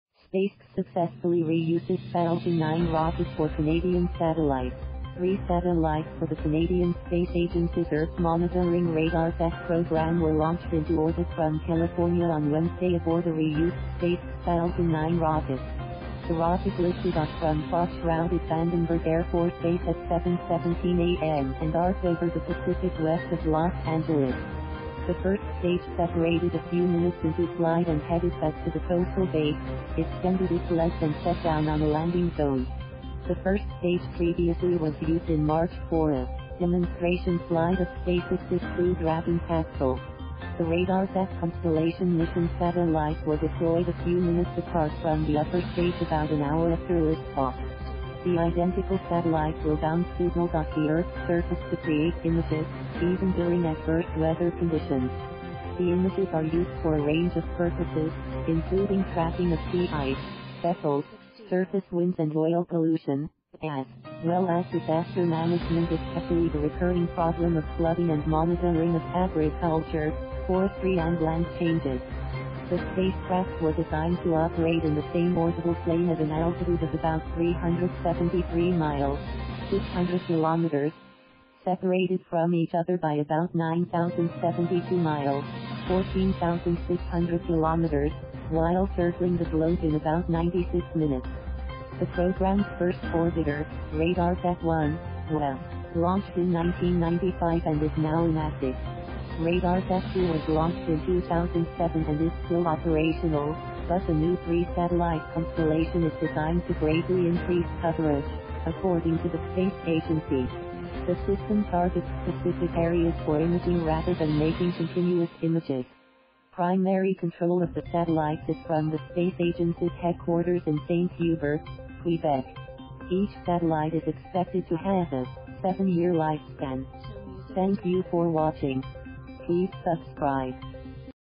SpaceX Successfully Reuses Falcon 9 Rocket for Canadian Satellites
Three satellites for the Canadian Space Agency’s Earth-monitoring Radarsat program were launched into orbit from California on Wednesday aboard a reused SpaceX Falcon 9 rocket. The rocket lifted off from fog-shrouded Vandenberg Air Force Base at 7:17 a.m. and arced over the Pacific west of Los Angeles. The first stage separated a few minutes into flight and headed back to the coastal base, extended its legs and set down on a landing zone. The first stage previously was used in March for a demonstration flight of SpaceX’s Crew Dragon capsule. The Radarsat Constellation Mission satellites were deployed a few minutes apart from the upper stage about an hour after liftoff. The identical satellites will bounce signals off the Earth’s surface to create images, even during adverse weather conditions. The images are used for a range of purposes, including tracking of sea ice, vessels, surface winds and oil pollution, as well as disaster management — especially the recurring problem of flooding — and monitoring of agriculture, forestry and land changes. The spacecraft were designed to operate in the same orbital plane at an altitude of about 373 miles (600 kilometers), separated from each other by about 9,072 miles (14,600 kilometers) while circling the globe in about 96 minutes. The program’s first orbiter, Radarsat-1, was launched in 1995 and is now inactive. Radarsat-2 was launched in 2007 and is still operational, but the new three-satellite constellation is designed to greatly increase coverage, according to the space agency. The system targets specific areas for imaging rather than making continuous images. Primary control of the satellites is from the space agency’s headquarters in Saint-Hubert, Quebec. Each satellite is expected to have a seven-year lifespan.